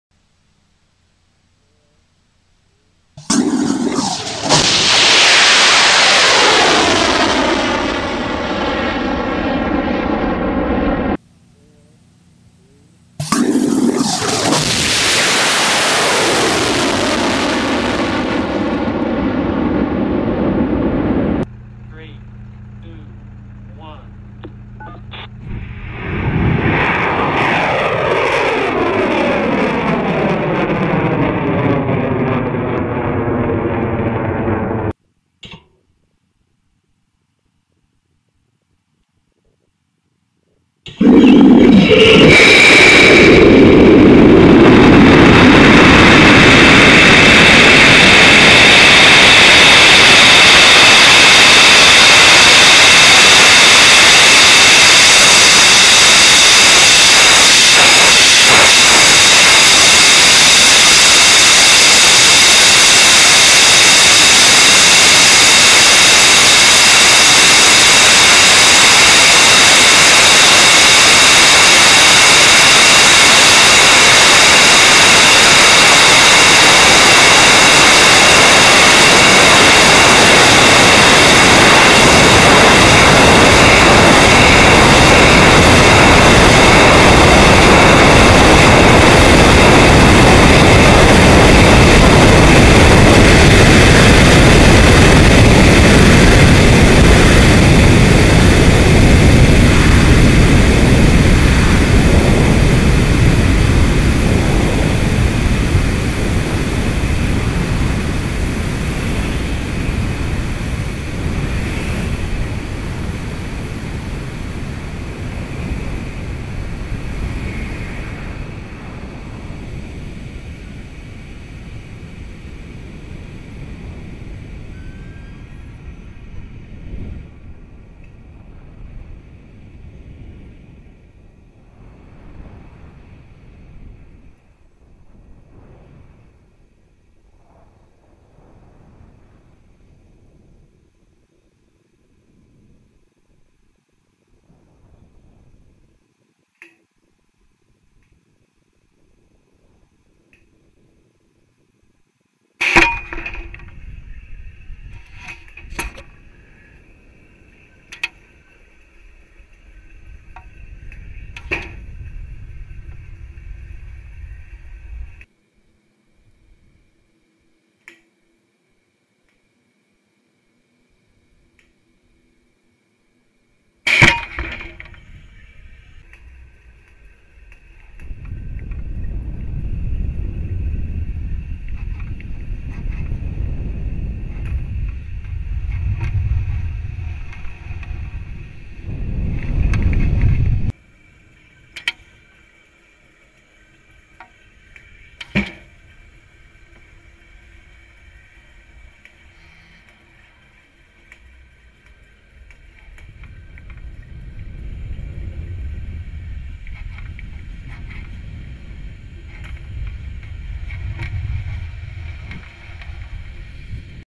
Armadillo Stiga rocket high altitude launch to nearspace
December 4 2011 launch of Armadillo Aerospace Stiga at Spaceport America. Apogee is near 140,000 ft above sea level.